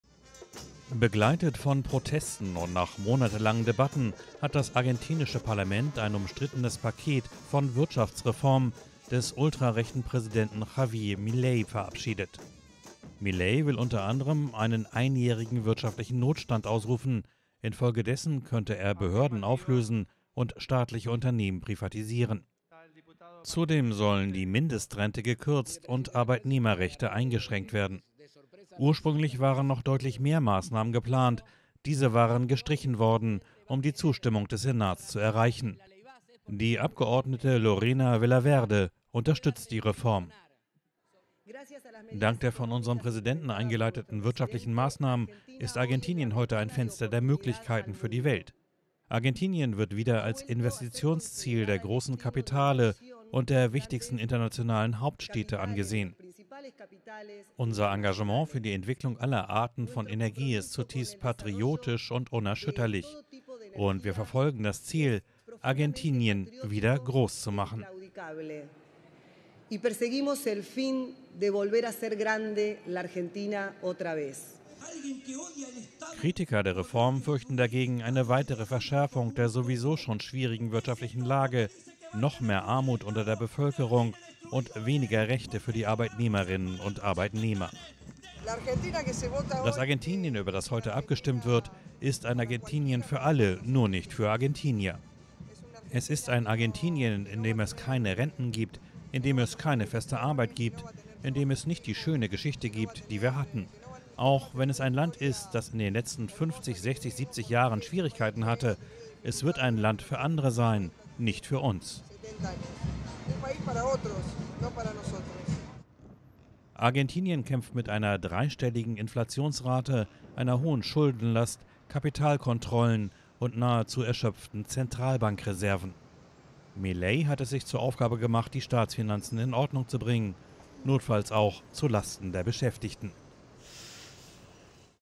Präsident Javier Milei bringt umstrittene Wirtschaftsreformen durch Kongress
Argentiniens Parlament hat die Wirtschaftsreformen von Präsident Milei verabschiedet. Dadurch können die Mindestrente gekürzt und Arbeitnehmerechte eingeschränkt werden.

Bildrechte Thumbnail: Tomas Cuesta/Reuters